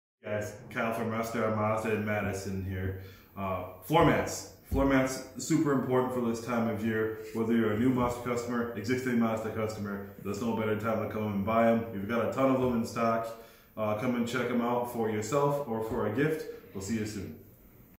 All Weather Floormats - Russ Darrow Mazda of Madison
Keep your Interior clean with all weather floormats!

Tune in with Kyle as he goes over our floormat options that are available. They are made to fit your specific vehicle and they come in different colors! DarrowDifference 🚗